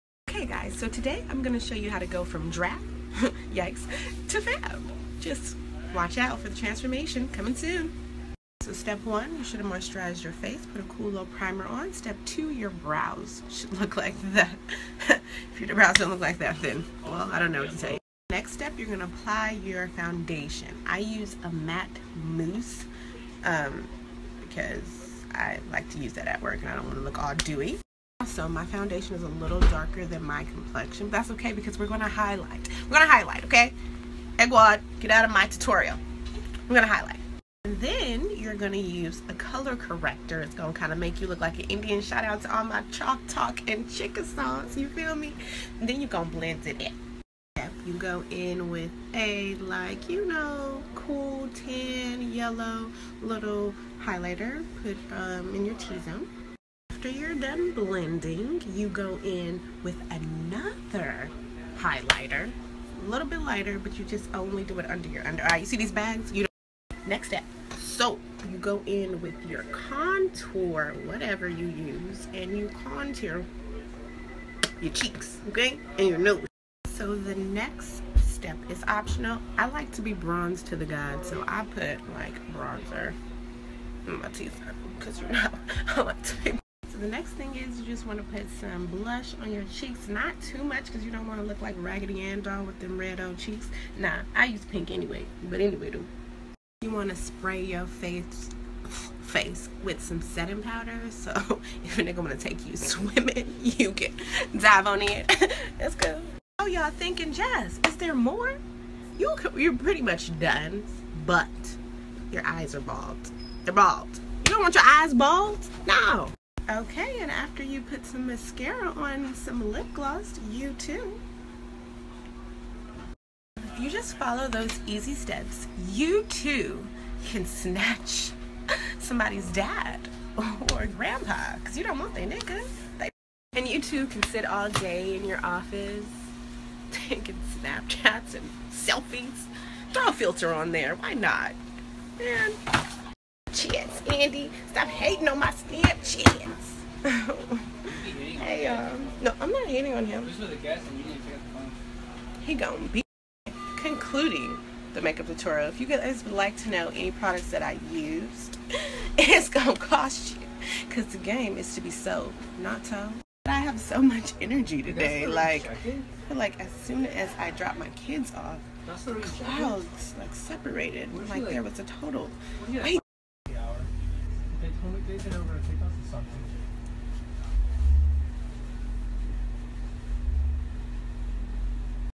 Mock Makeup Tutorial - Everyday makeup routine.
Please enjoy this mock makeup tutorial. I was playing around on Snapchat a few years ago and came up with this. I will post a real tutorial of how I do my makeup now. Hope you all get a laugh out of this! ;)